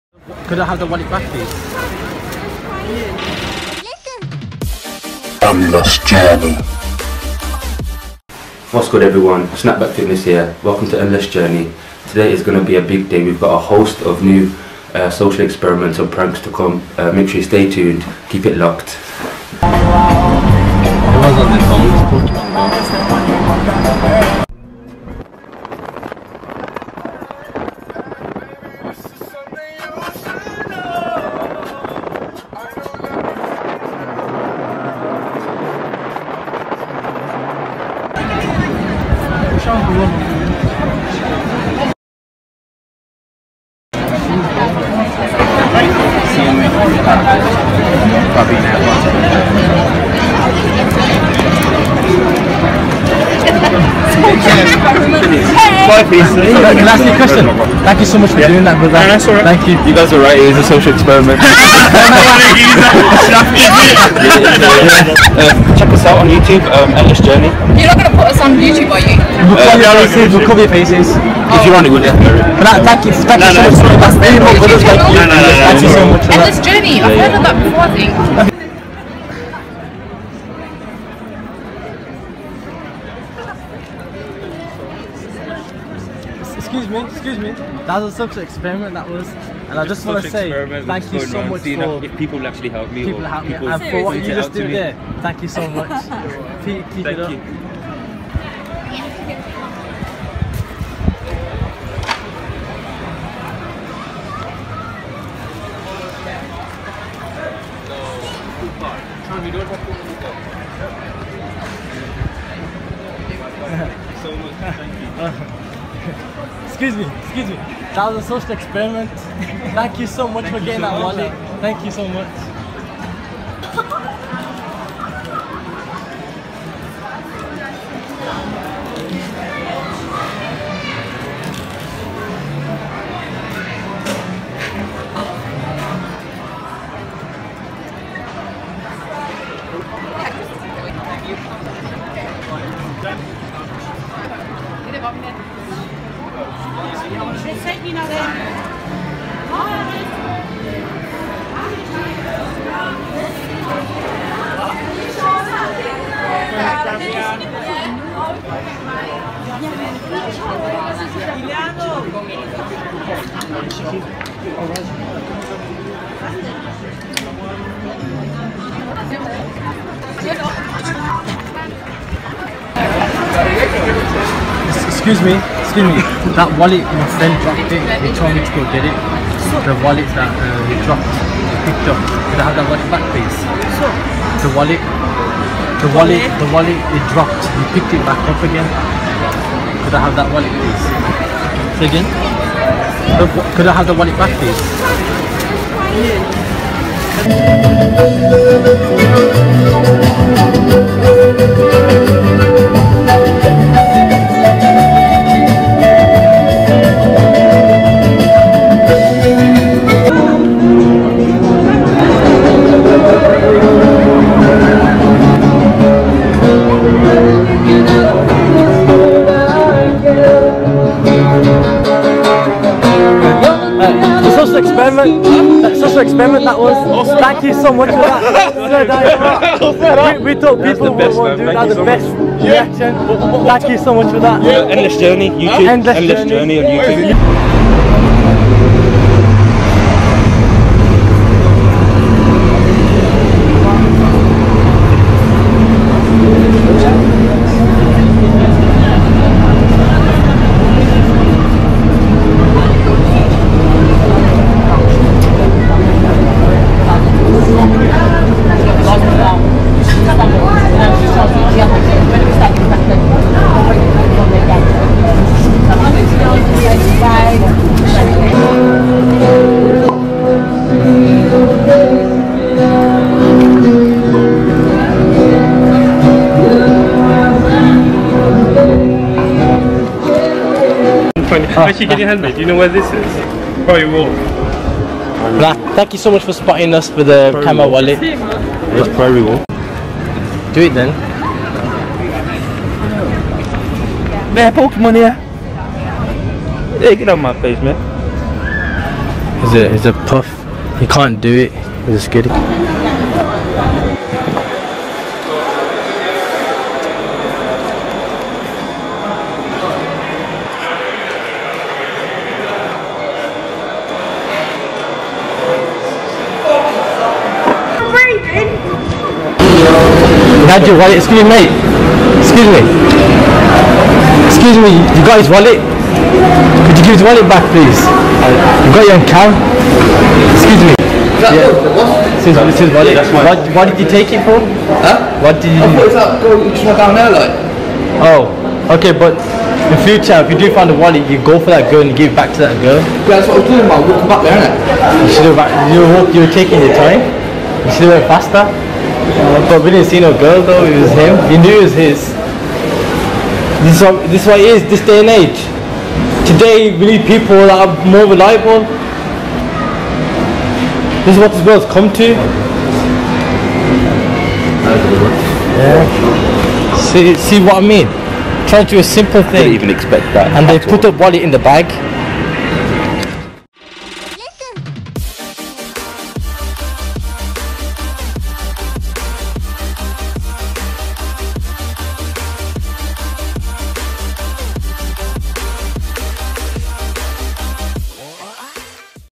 MUSLIM DROPPING WALLET IN PUBLIC SOCIAL EXPERIMENT - STOLEN!!
We decided to try the dropping wallet social experiment in the city centre dress in the traditional muslim dress code. we were very impressed and grateful for the help of the public. There is however a flip side to this. The wallet we had dropped was stolen. Watch for the full story.

follow: instagram -IG: Snapback_Fitness & IG:  LimitlessMoj